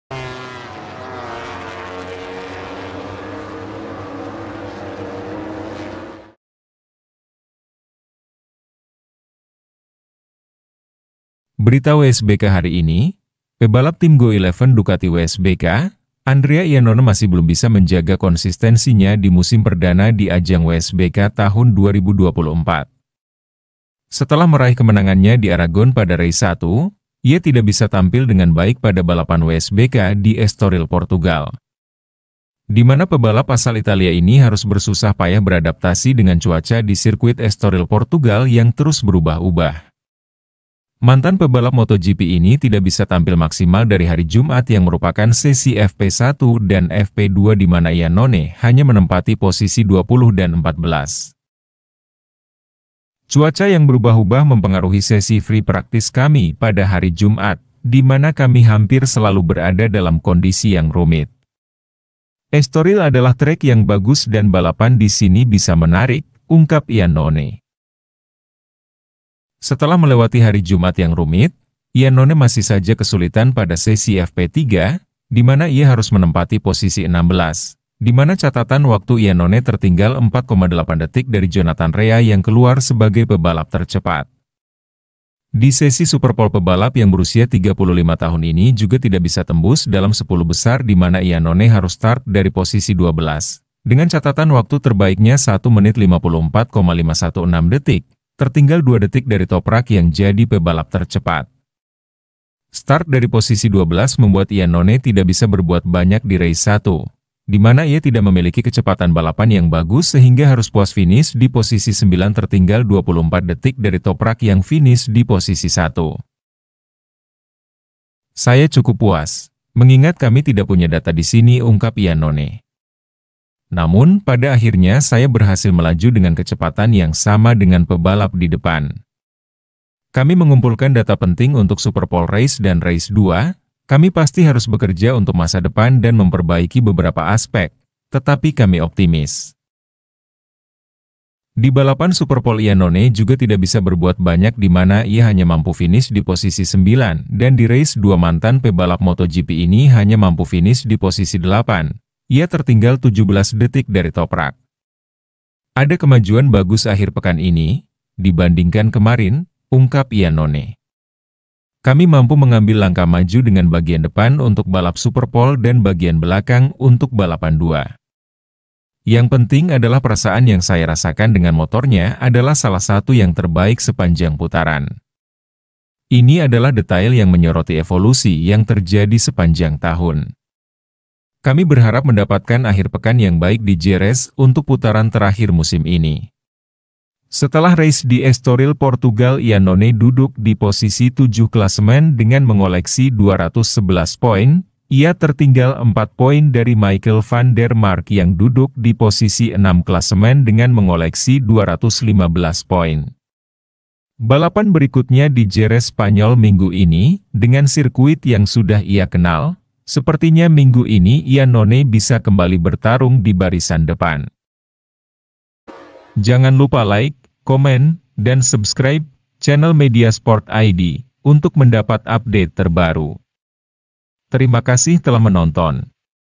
GAGAL BERSINAR DI WSBK ESTORIL PORTUGAL, KONSISTENSI IANNONE MASIH KURANG
BERITA MOTOGP HARI INI, GAGAL BERSINAR DI WSBK ESTORIL PORTUGAL, KONSISTENSI IANNONE MASIH KURANG

1 TOPRAK RAZGATLIOGLU
2 NICOLO BULEGA
3 ALVARO BAUTISTA
4 DANILO PETRUCCI
5 ALEX LOWES
6 MICHAEL VAN DER MARK
7 ANDREA IANNONE
8 ANDREA LOCATELLI
9 GARRETT GERLOFF
10 REMY GARDNER
11 IKER LECUONA
12 XAVI VIERGE
13 JONATHAN REA
14 SCOTT REDDING
15 AXEL BASSANI
16 DOMINIQUE AEGERTER
17 MICHAEL RUBEN RINALDI
18 SAM LOWES
19 NICHOLAS SPINELLI
20 TITO RABAT
21 BRADLEY RAY
22 ALESSANDRO DELBIANCO
23 TARRAN MACKENZIE
24 PHILIPP OETTL
25 Michele pirro
26 Markus reiterberger
27 IVO LOPEZ